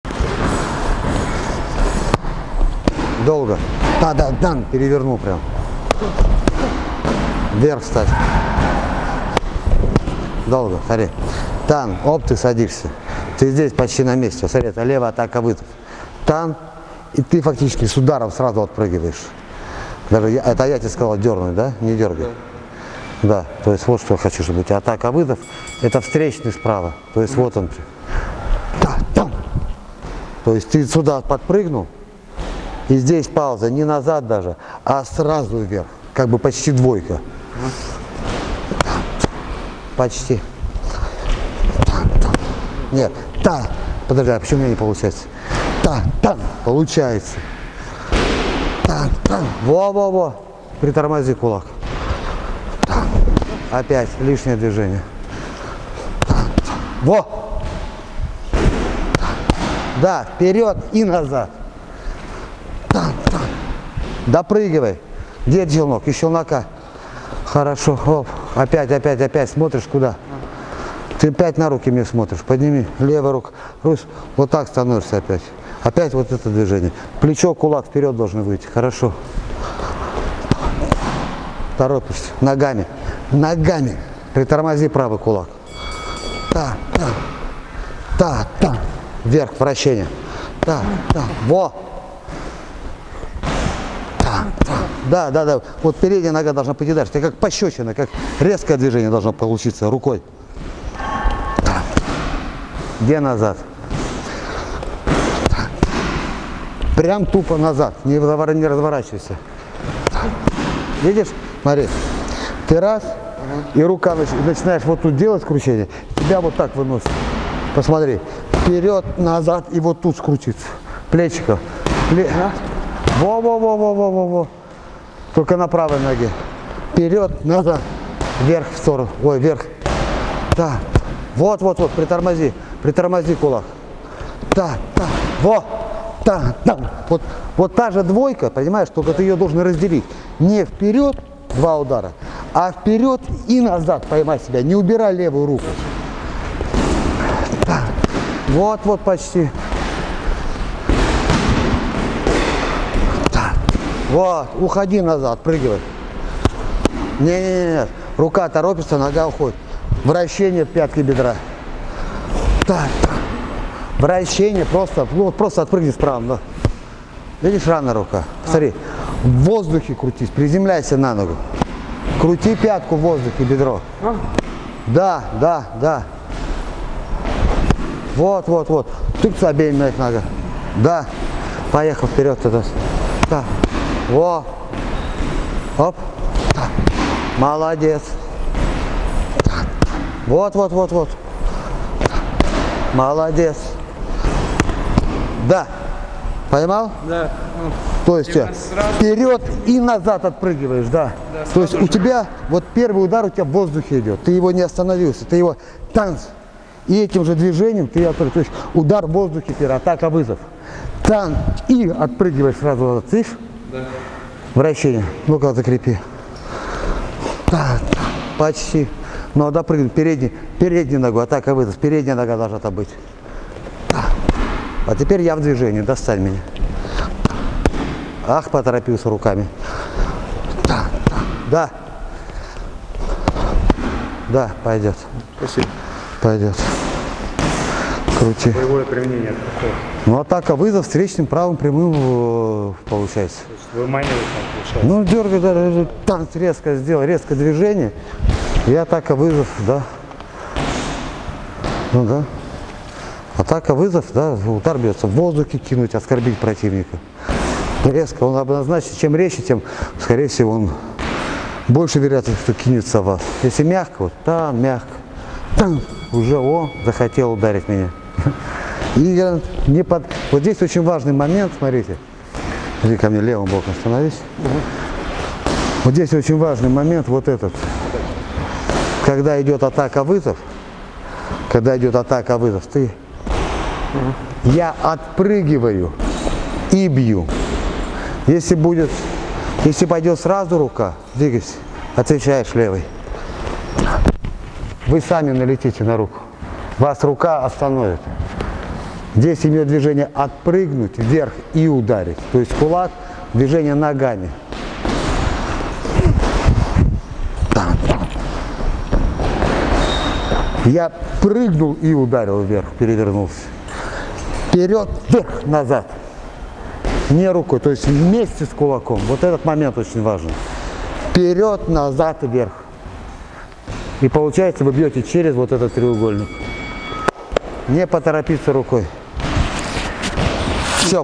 Бокс: разнонаправленная двойка (English subs)
Как выманить противника на правый прямой. Записывайтесь на занятия! т. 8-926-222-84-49. Где проходят тренировки О тренере Стоимость занятий